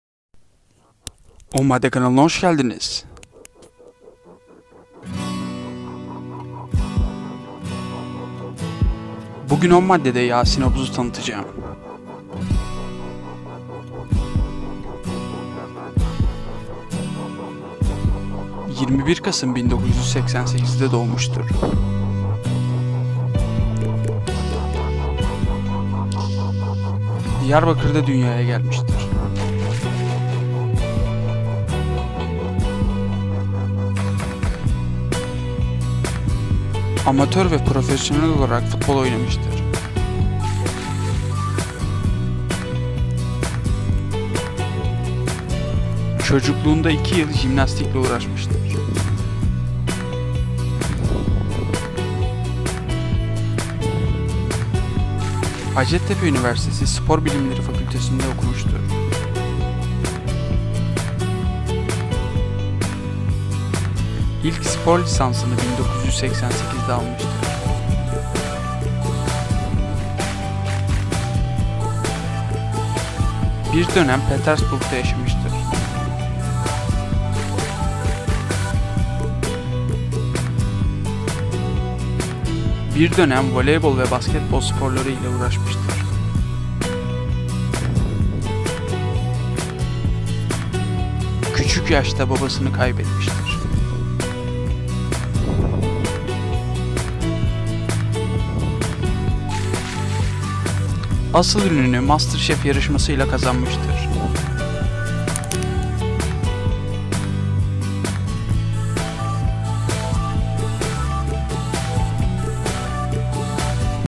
YASİN OBUZ KİMDİR VE NERELİDİR ? (10 MADDEDE YASİN OBUZ)
Bu günki videomda yasin obuzun hayatı ve yasin obuz hakkında bilinmeyenler ile alakalı bir video hazırladım.İyi seyirler.